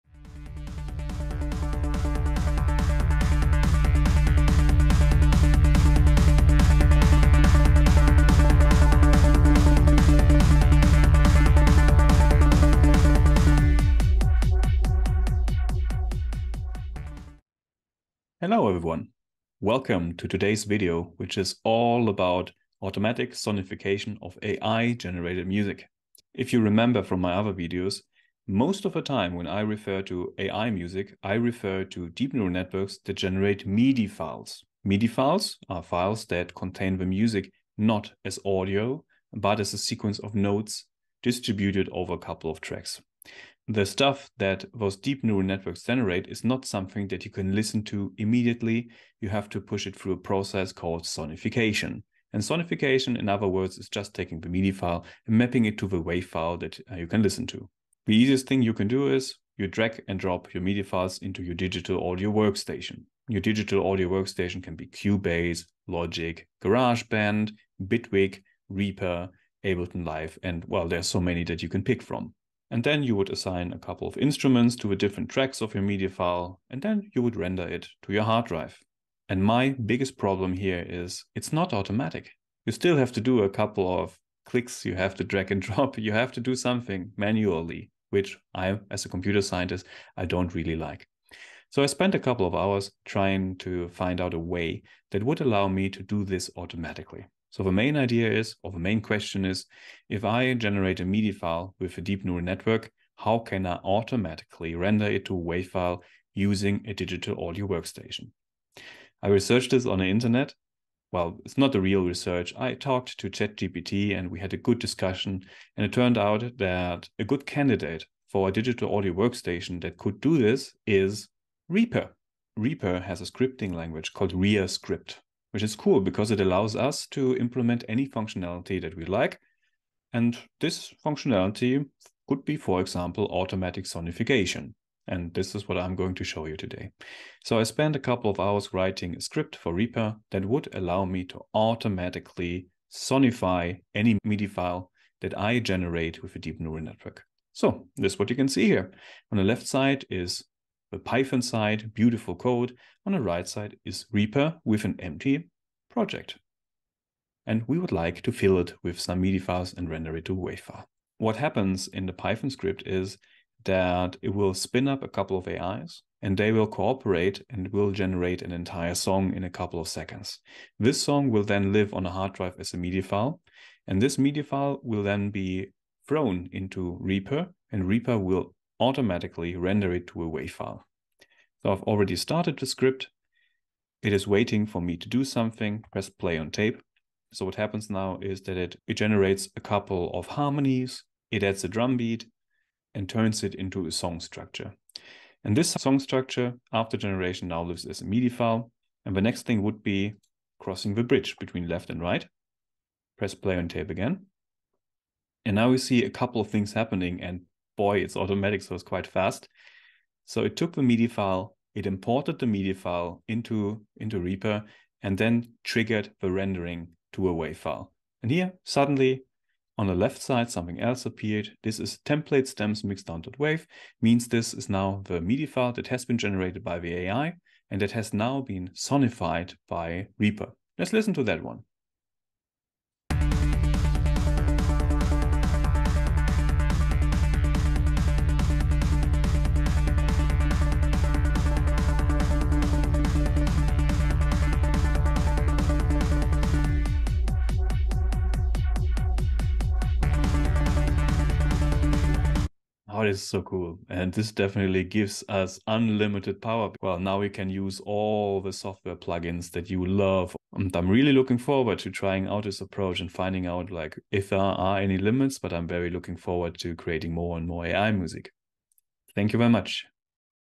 Automatic Sonification - MIDI to WAV with Python and Reaper DAW - AI Music
Reaper DAW is cool when you want to render MIDI to WAV. Especially if you want to do it automatically.